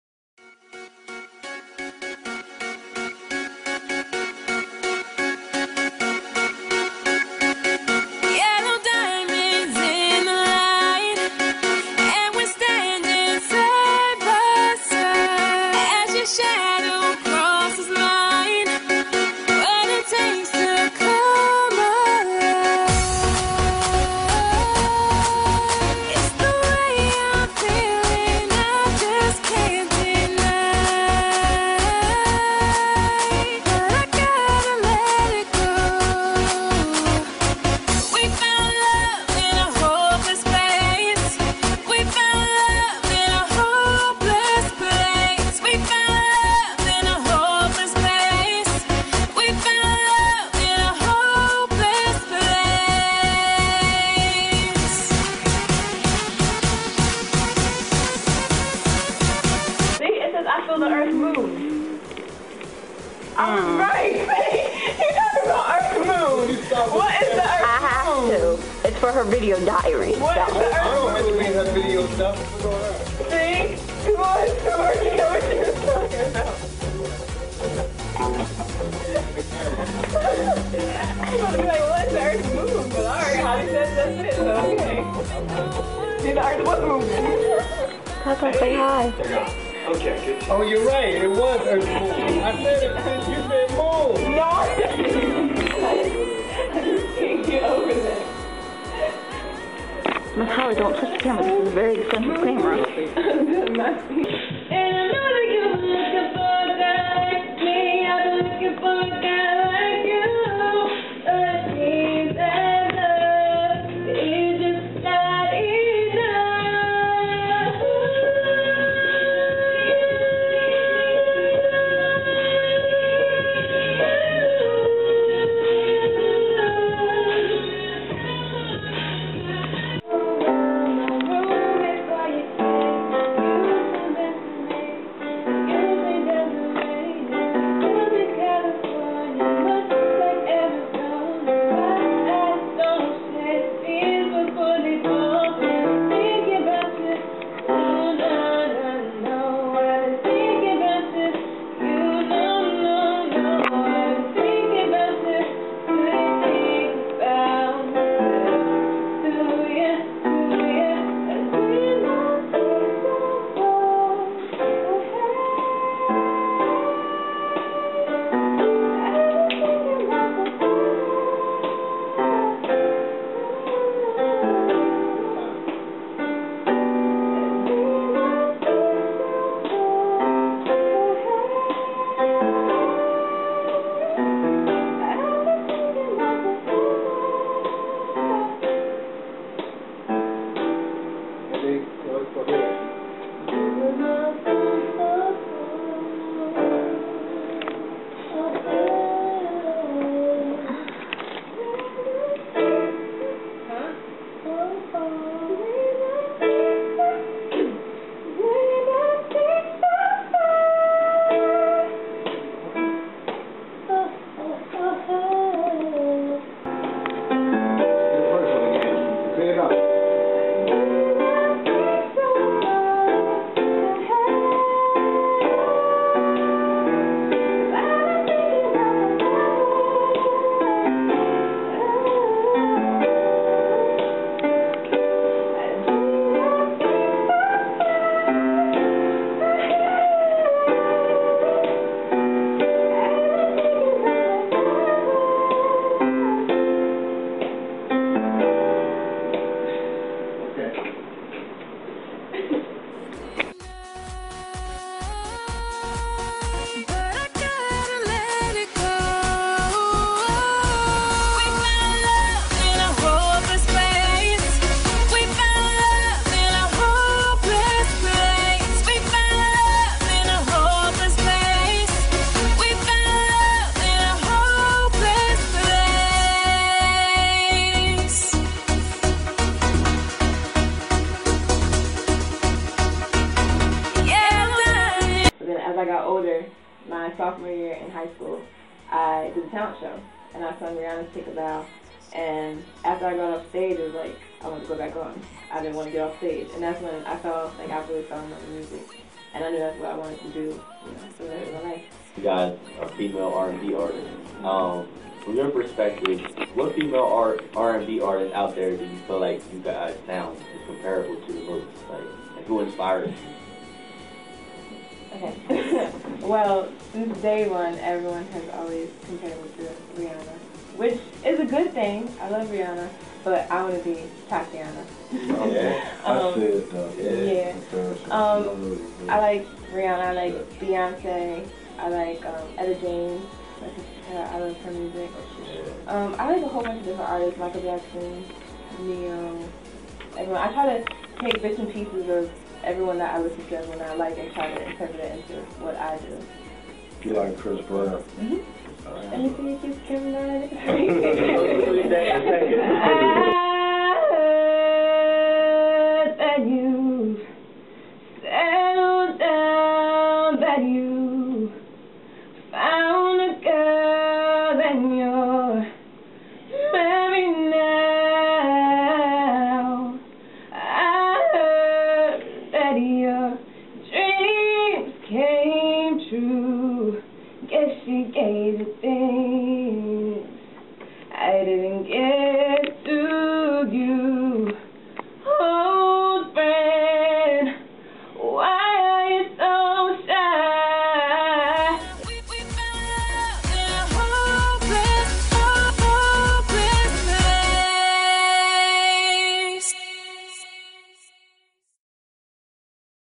"I Am Tati B" Vlog #1
Hey guys, Im gonna start doing these Vlogs so that you can get to know me a little better and see what I do on a daily basis. For my first episode you'll get to see me at artist development and also later tht night at Unsign Shine Radi0 1360 am wnjc1360.com